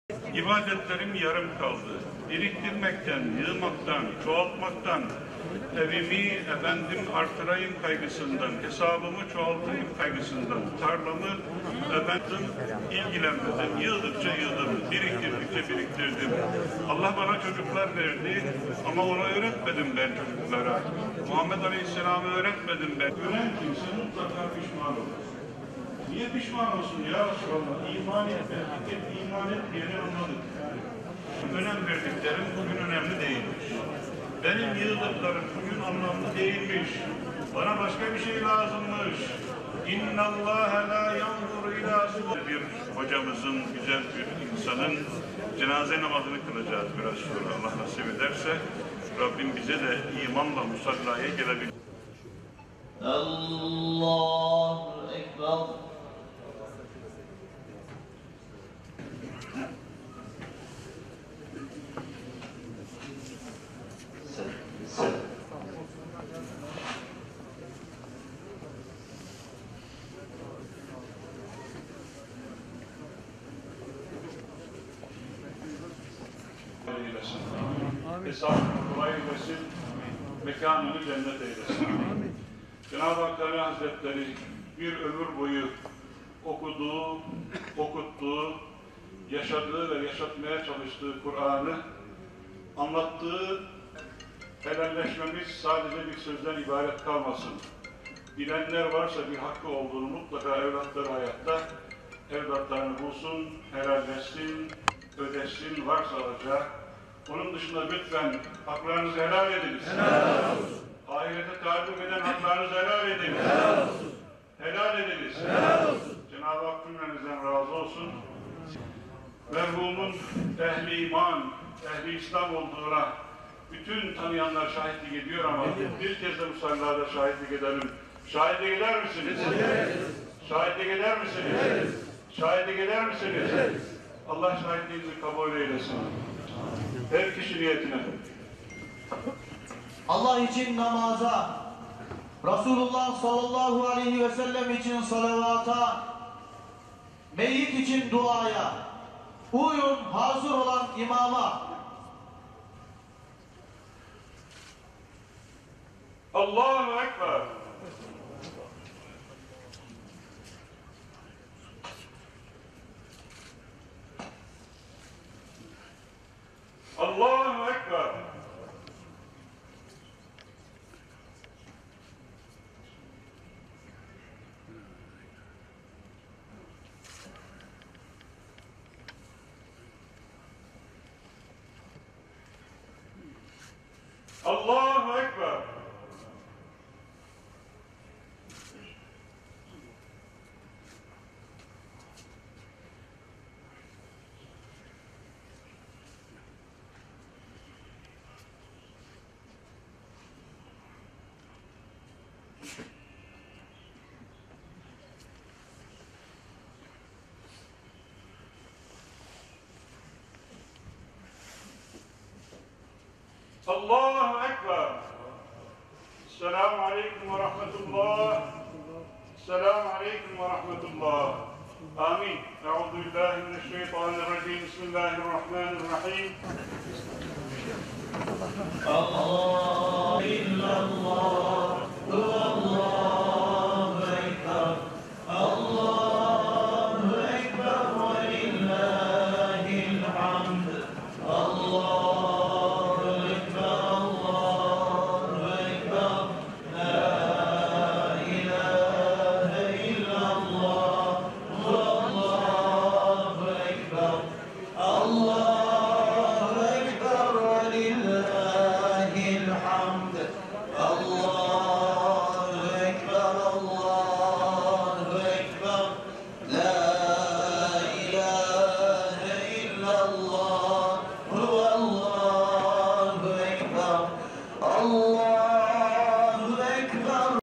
Eski Başbakan Yardımcısı Emrullah İşler’in babası son yolculuğuna uğurlandı
Eski Başbakan Yardımcısı Emrullah İşler’in vefat eden babası Abdullah İşler, Ankara Hacı Bayram Veli Camii’nde kılınan cenaze namazıyla son yolculuğuna uğurlandı.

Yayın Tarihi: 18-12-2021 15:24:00